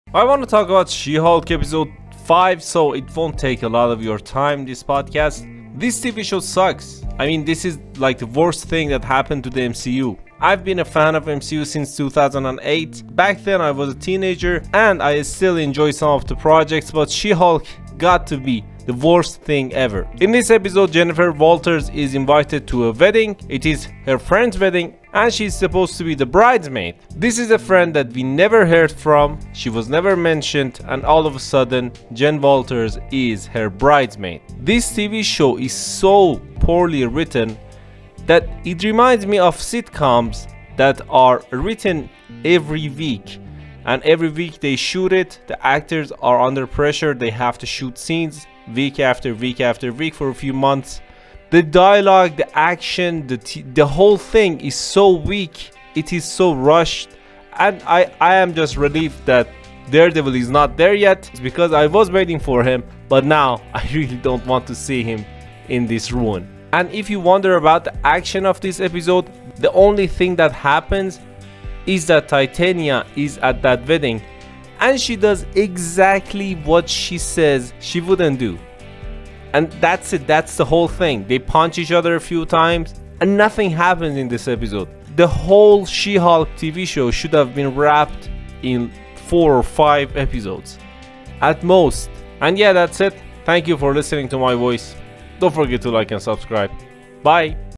She Hulk Episode 6 Review
It was obvious that this MCU and Disney Cooperation would suck after the second episode but it kept us all watching just to see Matt Murdock! I hope Daredevil shows up in the final episode of SheHulk and then leave soon because I really don't want him ruined.
What were your thoughts on She Hulk Episode 6?
Will you watch the final 3 Episodes of the show or you'll drop it?
Thanks, Bye!